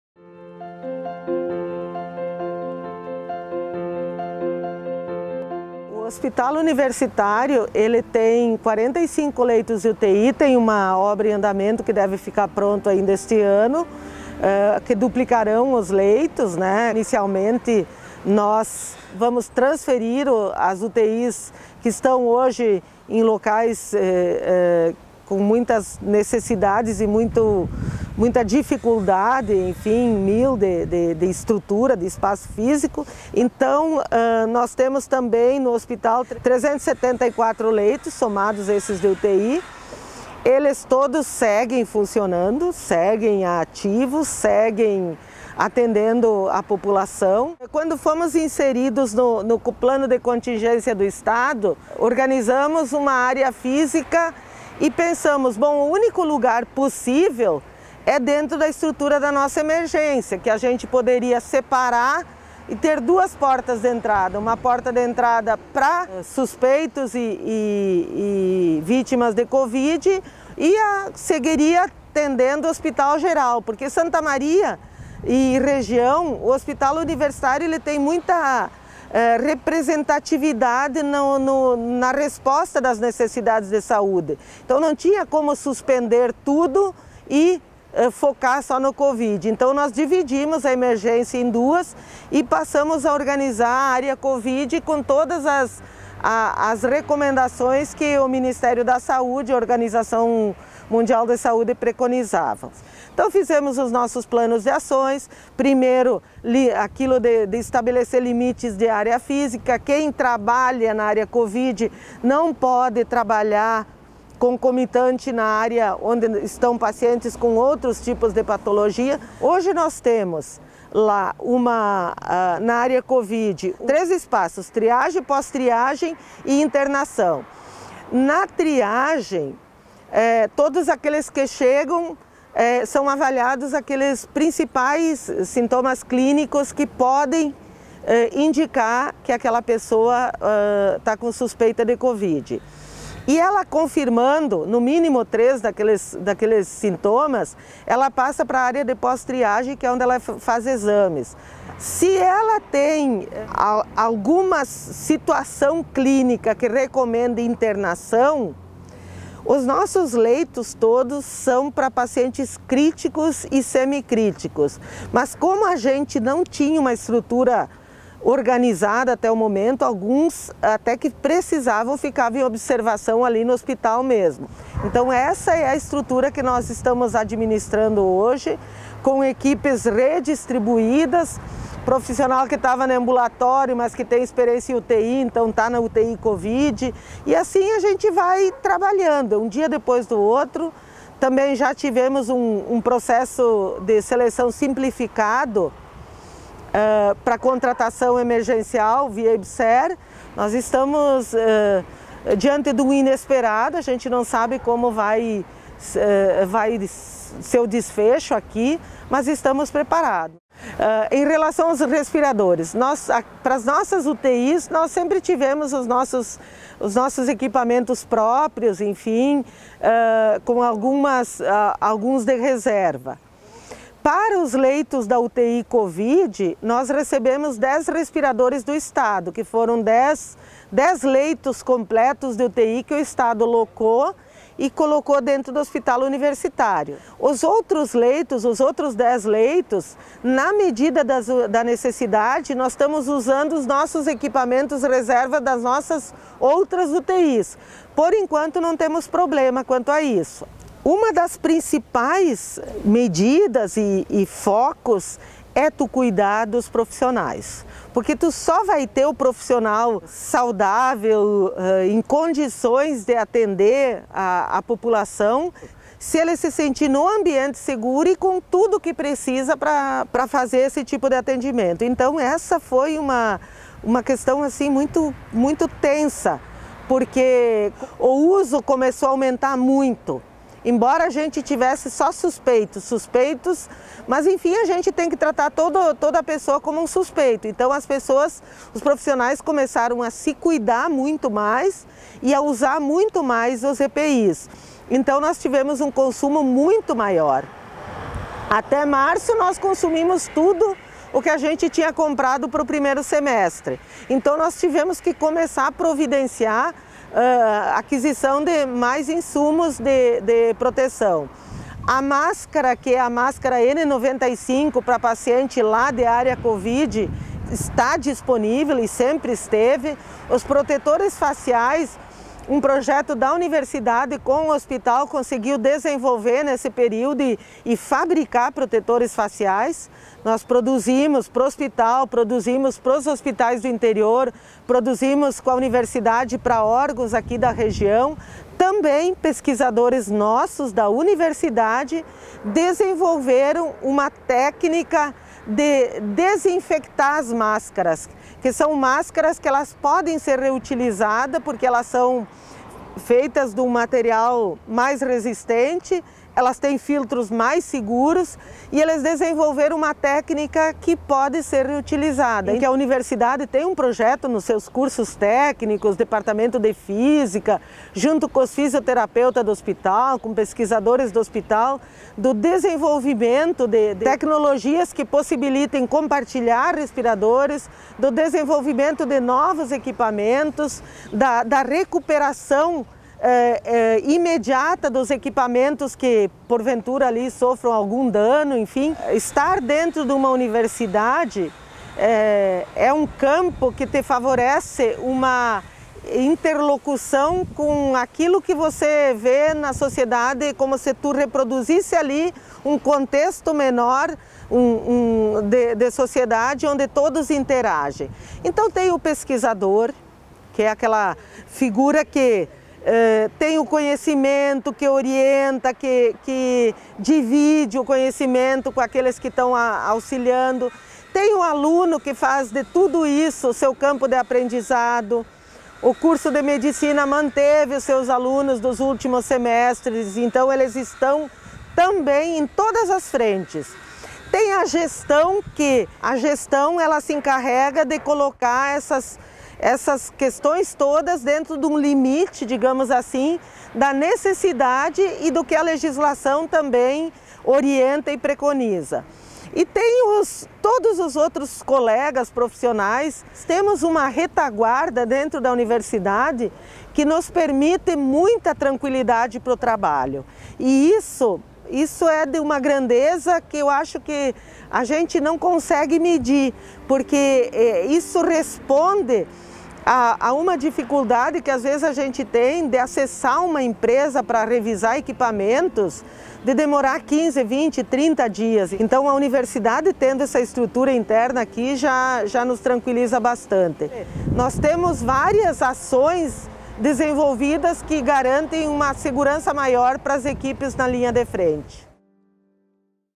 Situação do atendimento de casos de COVID-19 no HUSM
Como o Hospital Universitário de Santa Maria está atendendo os casos suspeitos e confirmados de COVID-19? Quantos leitos estão destinados a pacientes com a doença? Como está a situação de EPIs e respiradores?
A Gerente de Atenção à Saúde do Hospital traz essas e outras informações.